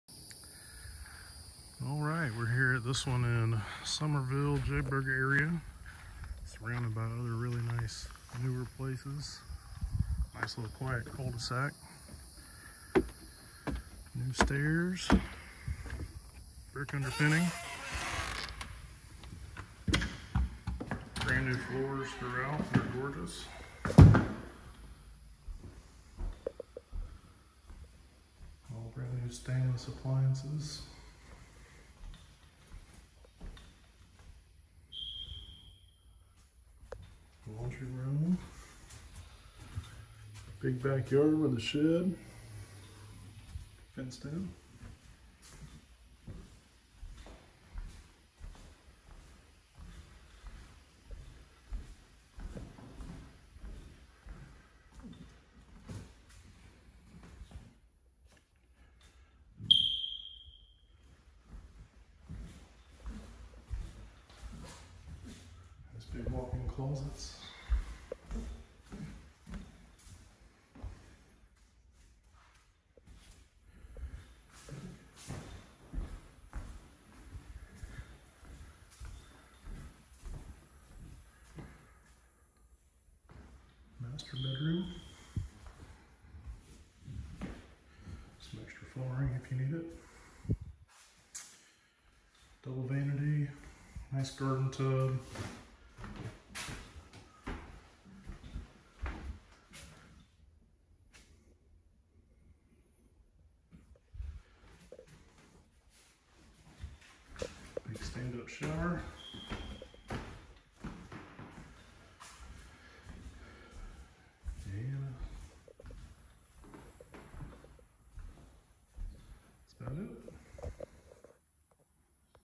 We Buy Houses Charleston - Walkthrough of a 3BD 2BA DWMH in Summerville
Walkthrough of a 3BD 2BA DWMH in Summerville on August 2nd, 2021.

We buy houses in Charleston, South Carolina, and surrounding areas.